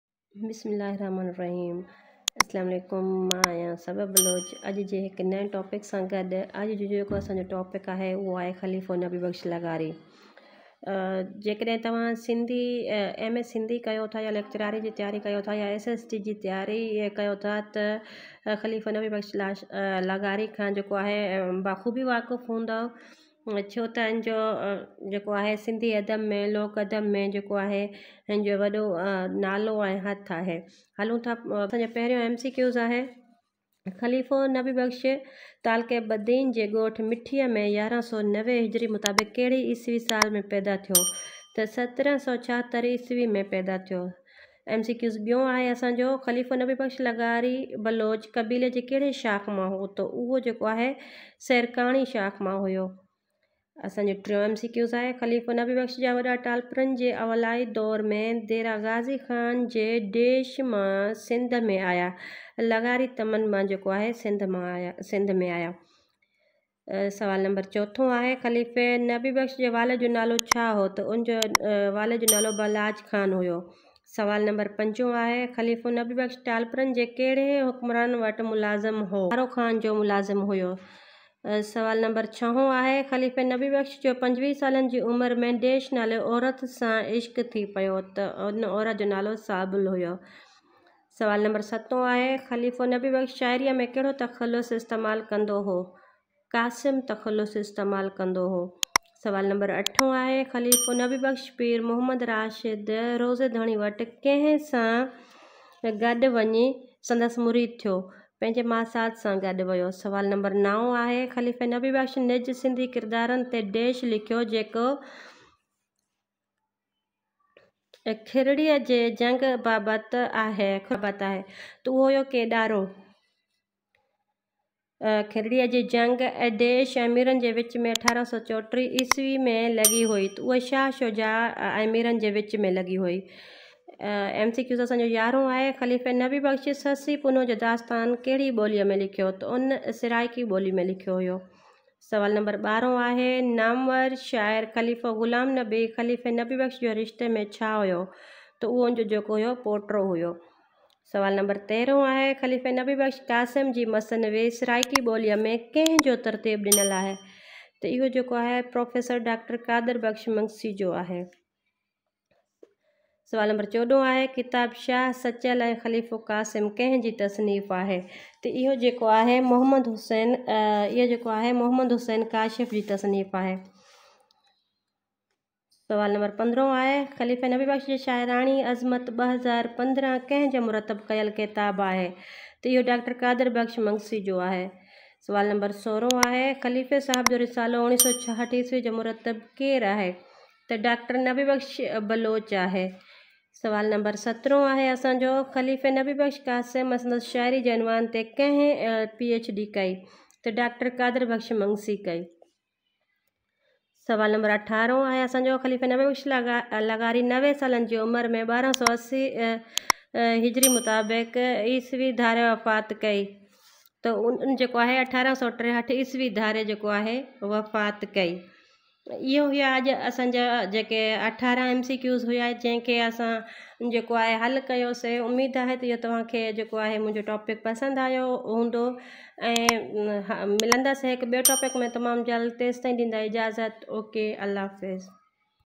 خليفو نبي بخش لغاري بلوچ|| بالاچ خان || ڪيڏارو|| sindhiadab2642@
خليفو نبي بخش لغاري بلوچ شاعري ۾ تخلص قاسم خليفي نبي بخش جي بابت 18 اھم معروضي سوالن جا جواب ليڪچراري جي لکت واري ٽيسٽ لاء اھم ويڊيو.

@ sindhiadab2642#youtube